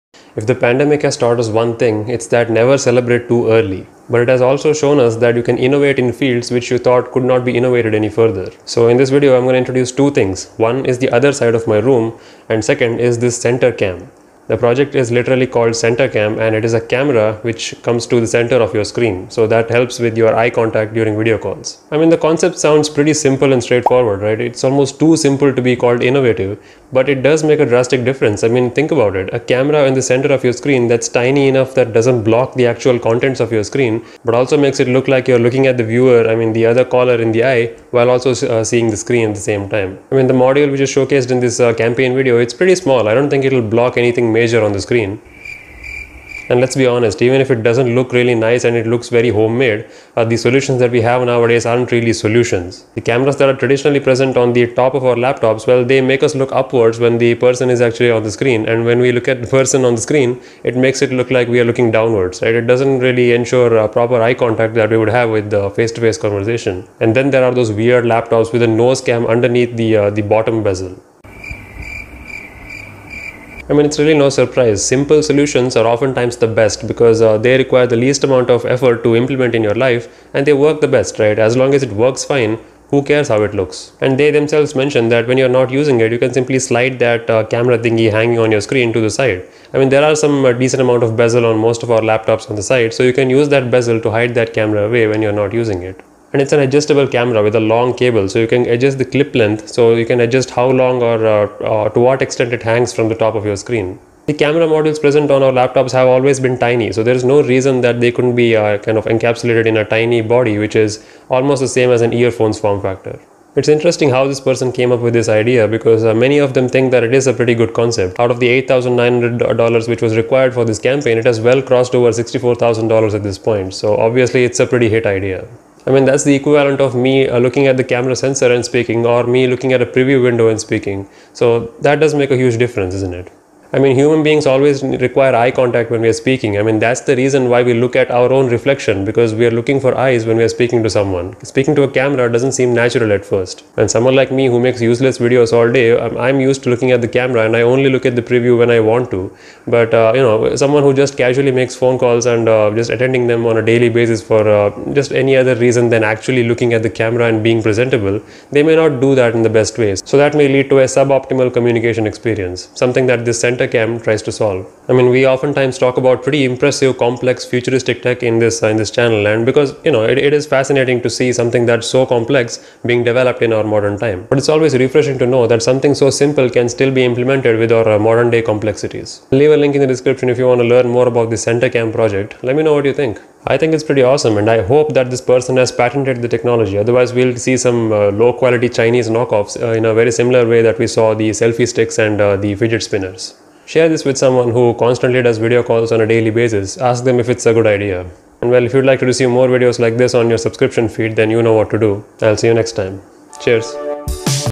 This Webcam helps you make eye contact in Video-calls!
This simple webcam solution called Center-cam helps you make eye contact during video calls!

Cheers!